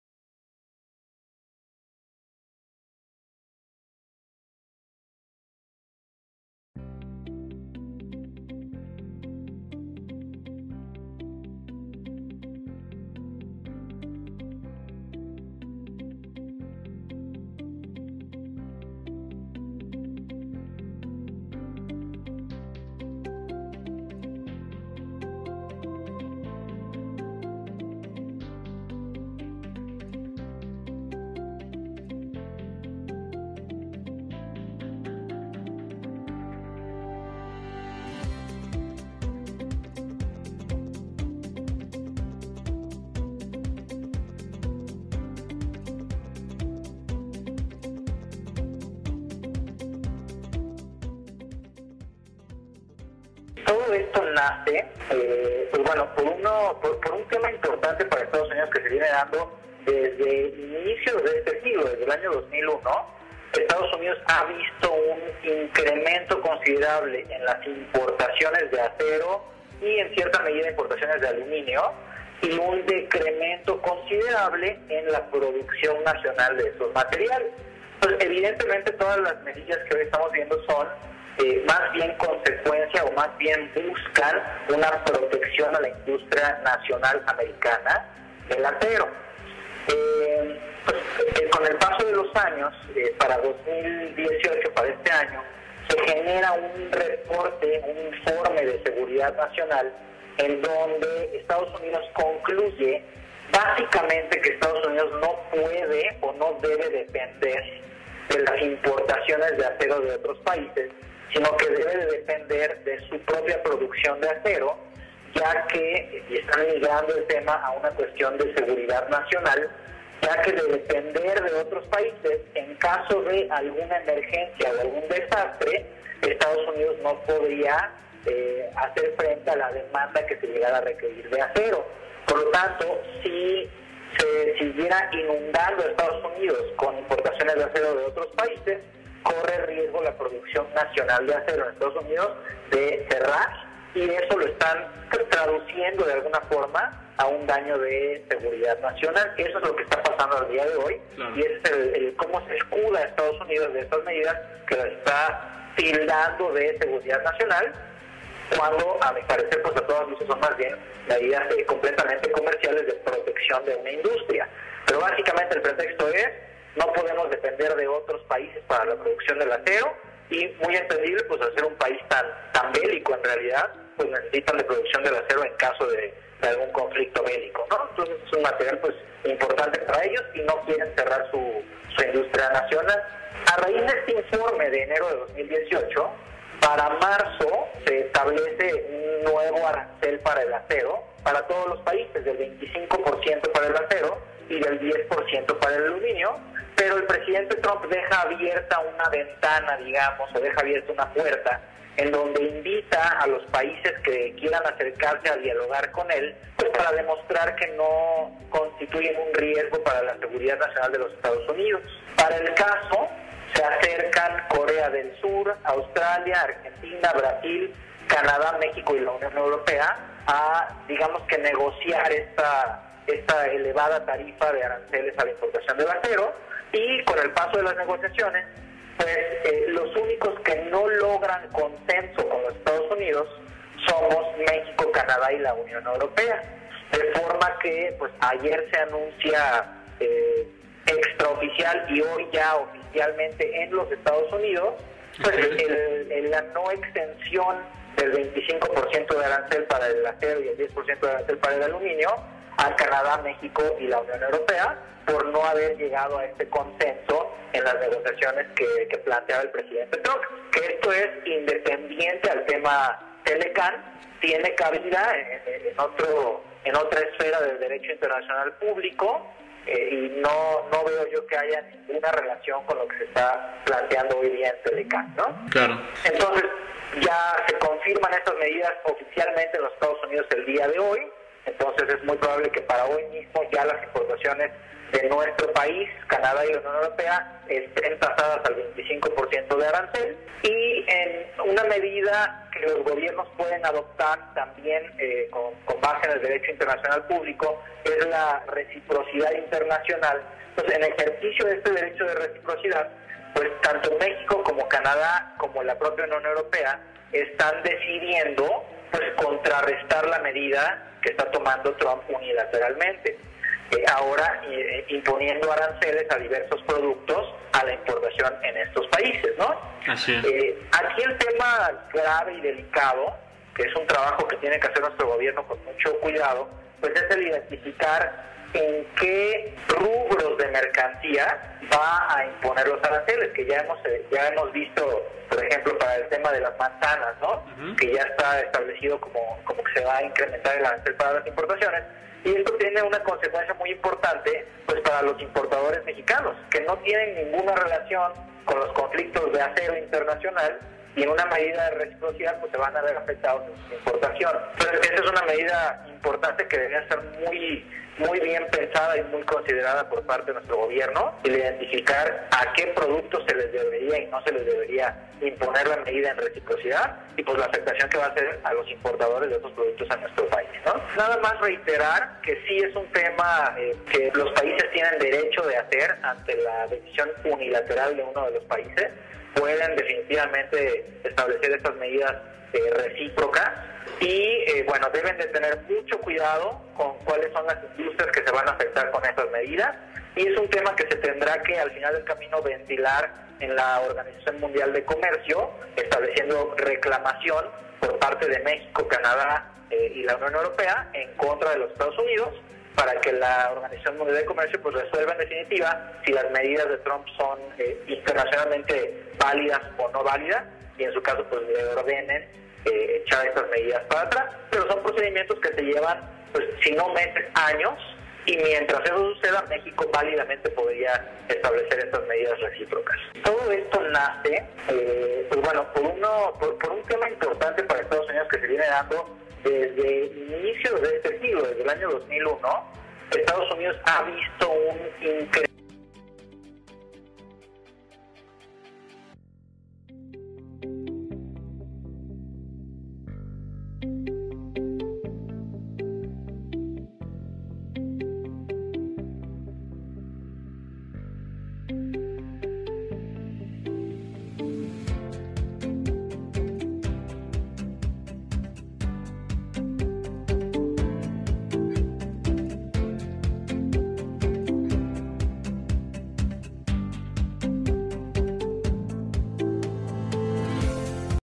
¿Por qué USA impuso aranceles al acero y aluminio? [Audio]
En esta colaboración para la revista Estrategia Aduanera, el Mtro. Oscar Rueda nos pone en contexto de lo que está pasando en la batalla de aranceles entre Estados Unidos, México, Canadá y la Unión Europea.

Entérate de estas cápsulas EN DIRECTO vía Twitter: @CJAduanero o en nuestro Boletín
Agradecemos tu retroalimentación de esta cápsula con un comentario o like!